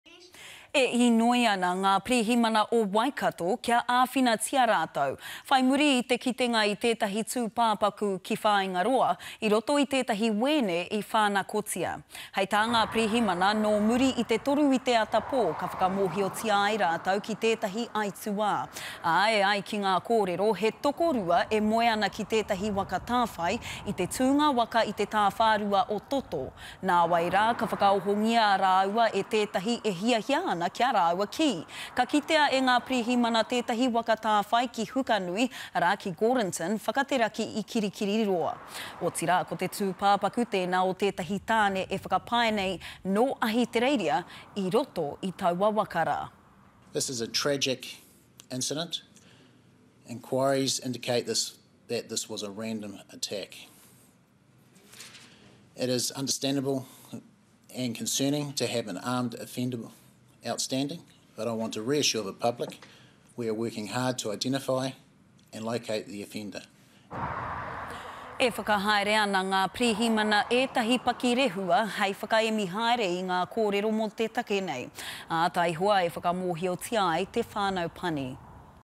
Waikato police appeal for information
Waikato police are appealing for information after locating a stolen van with the body of a man inside near Raglan. Police say they were notified of a serious incident just after three am. It's been reported that a couple were asleep in the caravan in a carpark at Toto Gorge before they were woken up by a person wanting the keys to the vehicle. Police then located the stolen rental caravan in Gordonton, northeast of Hamilton with the body of man, believed to be an Australian tourist. Police are conducting inquiries to establish the circumstances and are working to inform the next of kin.